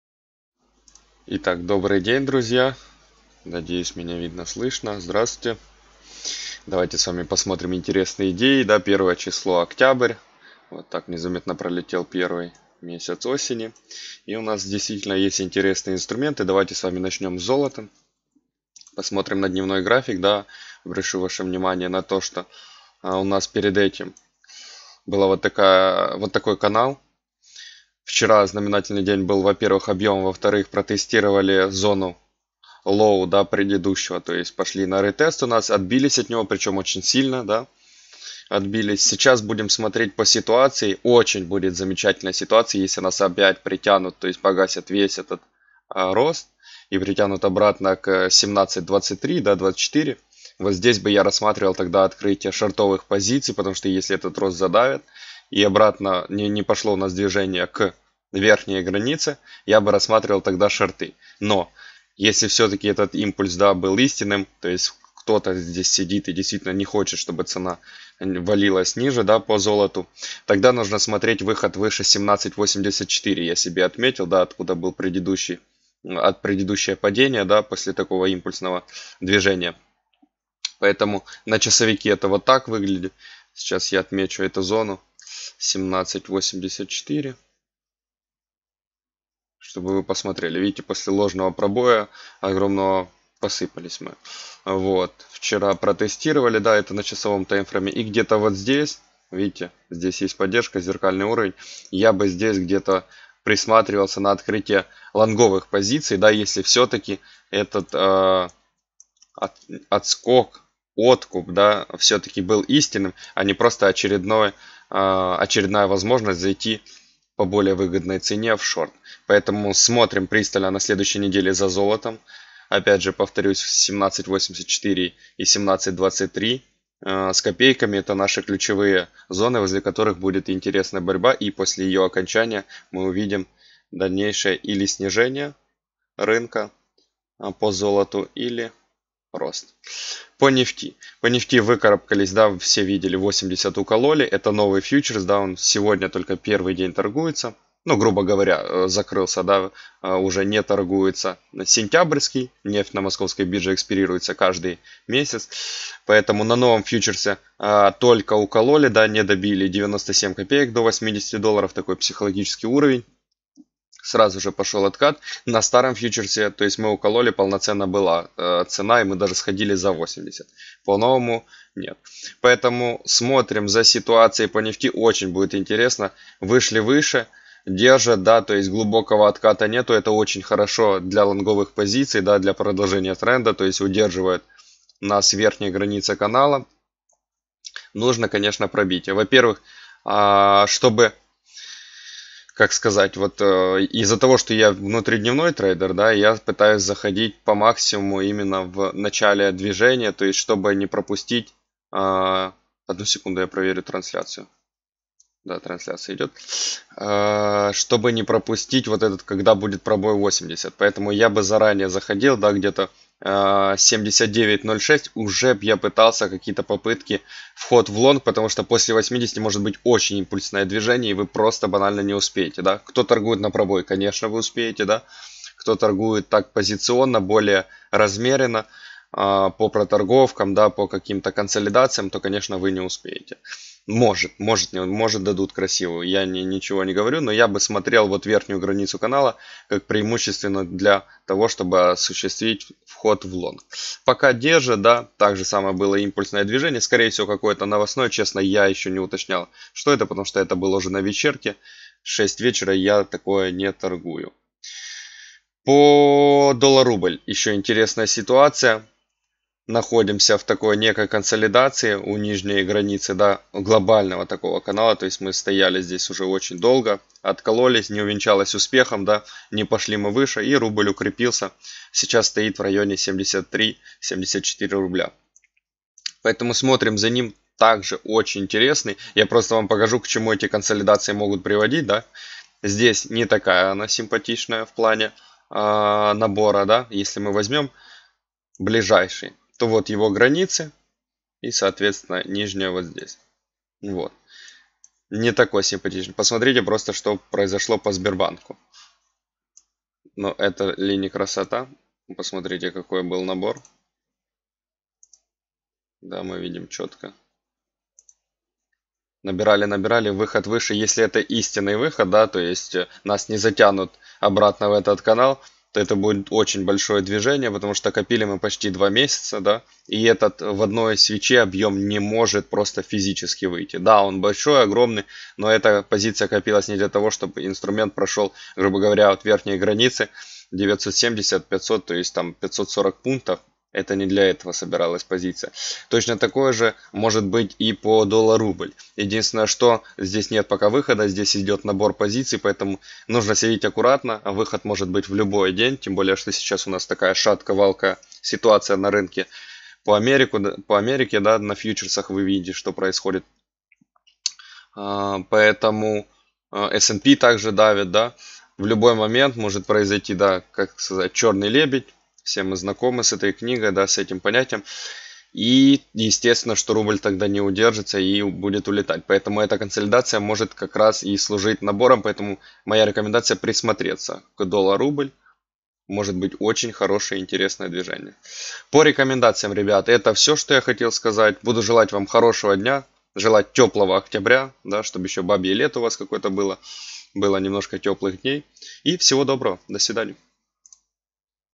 Дмитрий Черепащук: Октябрь обещает быть щедрым на движения по рынку | AMarkets
В рубрике “Открытый микрофон” Дмитрий Черепащук из Киева, который зарабатывает на финансовых рынках с 2015 года. Он занимается внутридневной торговлей на Московской бирже.
С 2019 года Дмитрий управляет капиталом от компании Forts life, демонстрируя среднюю доходность по счету 10-12%.